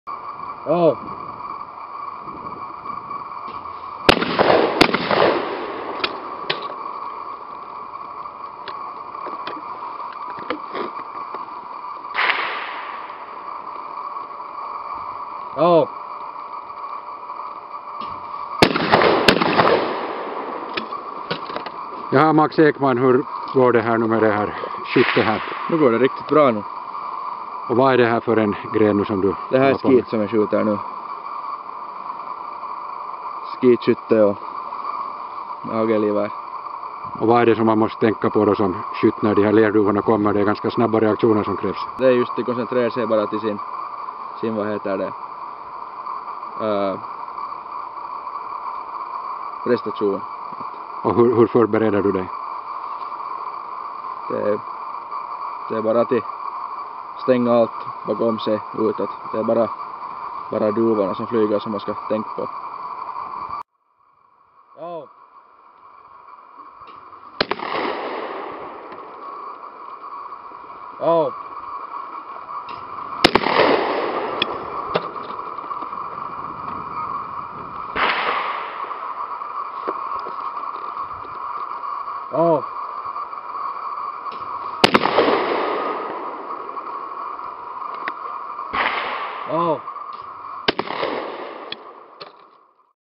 Skeetskytte
Max Ekman från Pargas tränar skeetskytte på skjutbanan i Pargas.